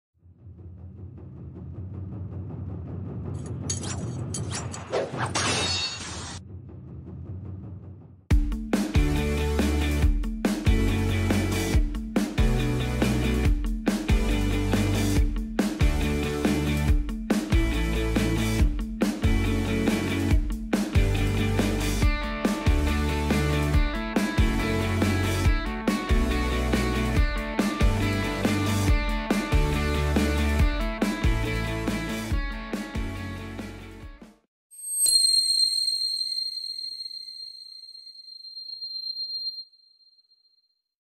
Taikai Revival 01 Ausweichtraining
Taikai Frankfurt a.M. Soke Furuta Revival Seminar

Sparringübung: Ausweichen mit Oberkörper ohne Deckungsarbeit

Oniguma Dojo Regensburg
Techniken nur unter Aufsicht eines autorisierten Trainers Bujinkan Budo Taijutsu ausführen.
Perform techniques only under the supervision of an authorized Bujinkan Budo Taijutsu Instructor.